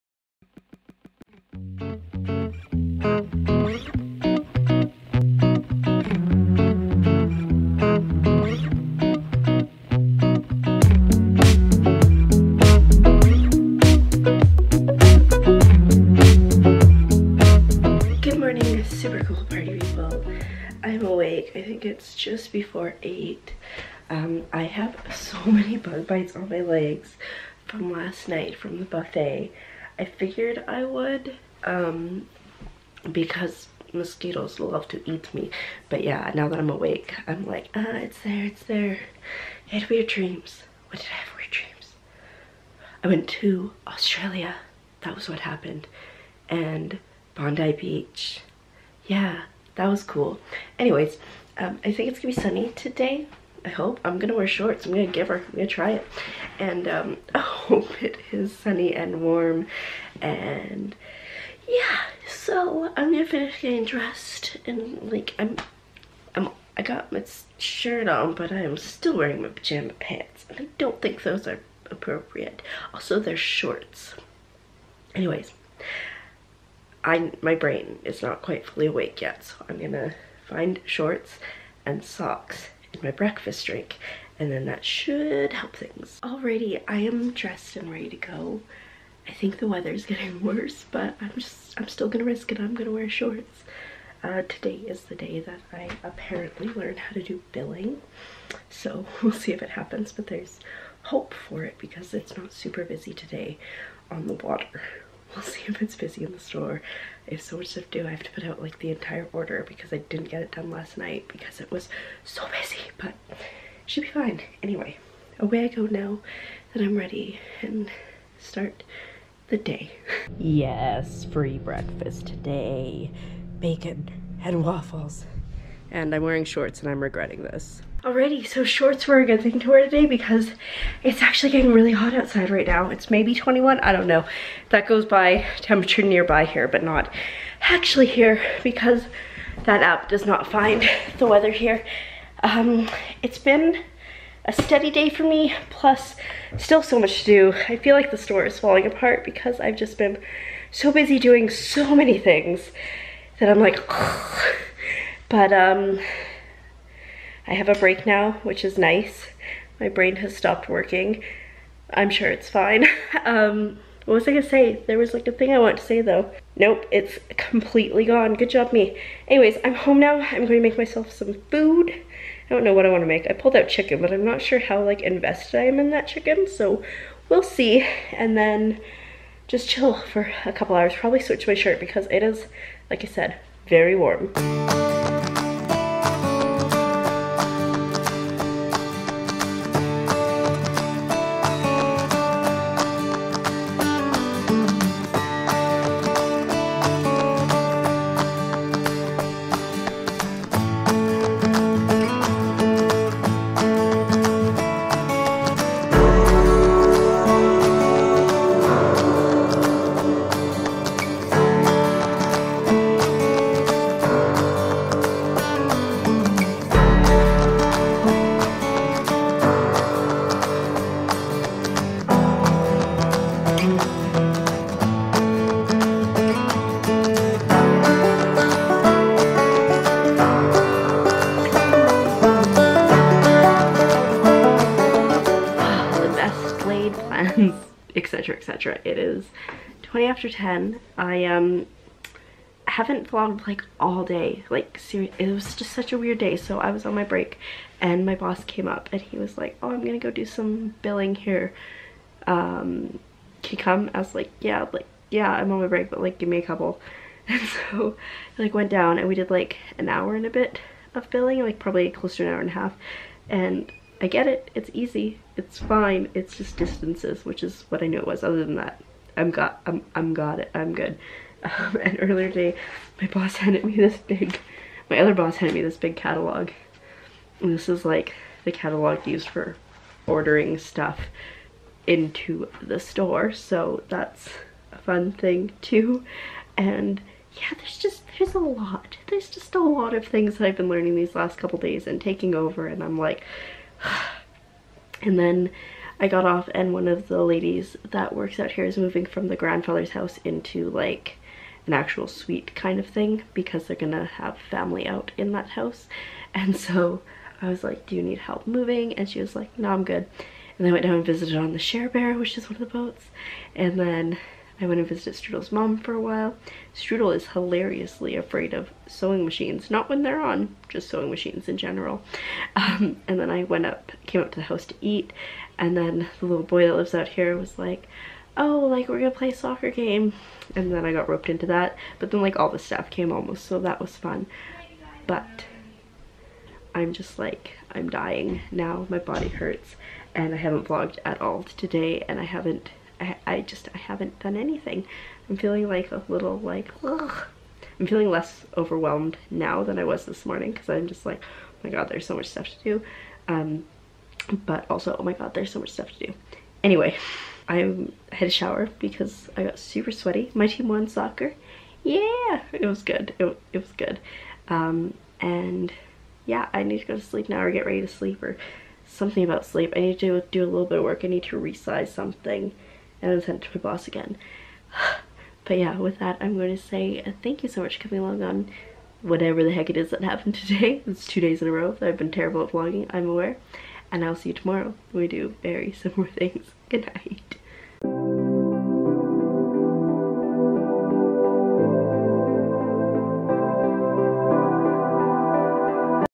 Free Breakfast & A Soccer Game | July 13, 2019
I’m slowly getting food from the kitchen, which is really exciting, and then my team won an awesome soccer game.

Equipment:
Camera: Canon G7x Mark II
Tripod: Manfrotto MTPIXI-B PIXI Mini Tripod
Editing Software: Filmora